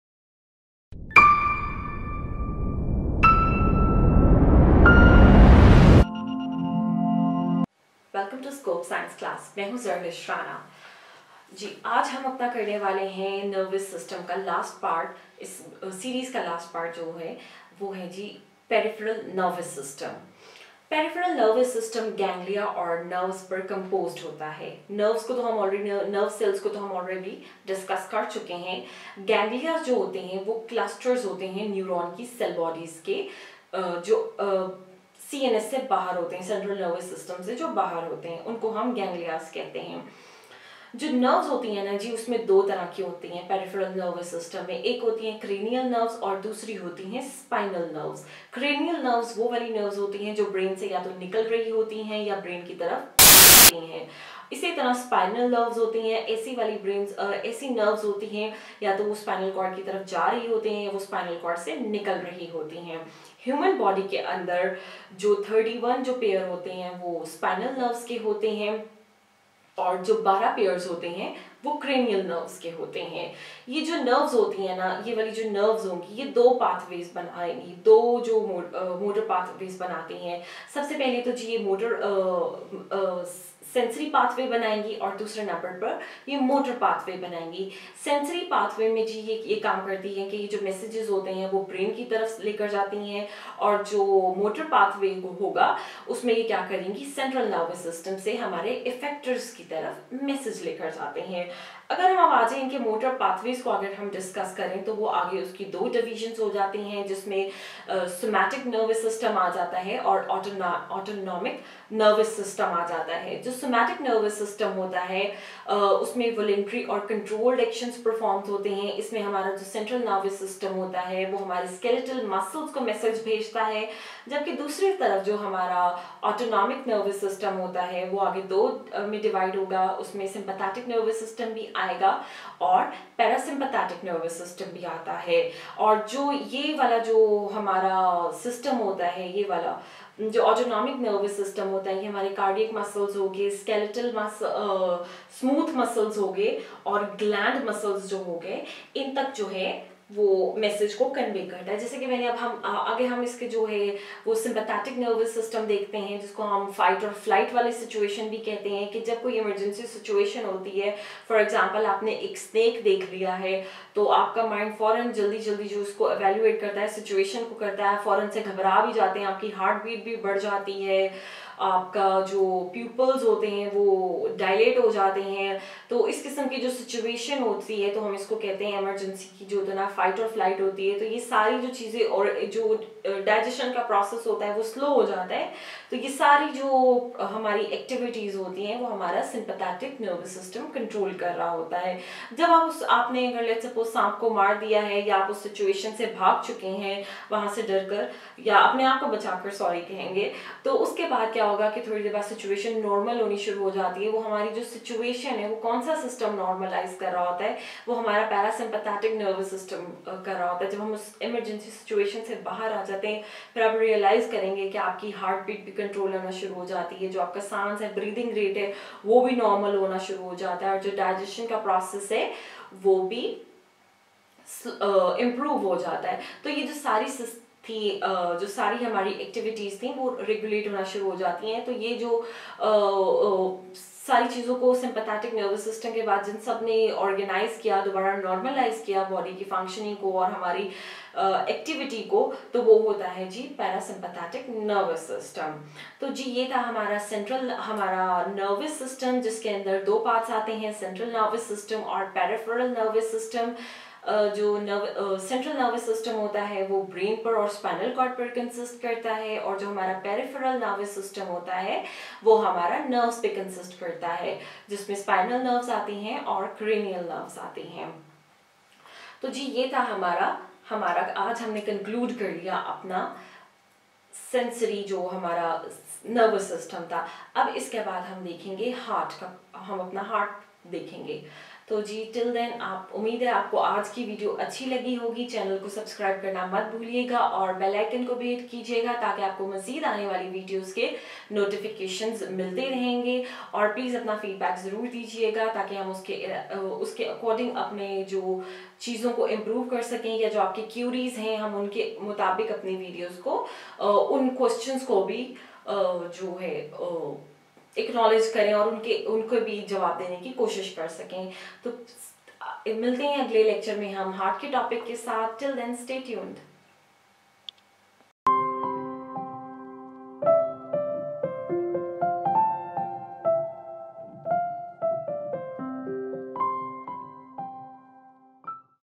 CSS General Science: Peripherel Nervous System
Miss Zarlish Rana is a professional Pharmacist. She holds professional degree in Medicine BZU, Multan. She has been associated with Highnoon Laboratories. She carried an out standing result in the paper of CSS General Science & Ability.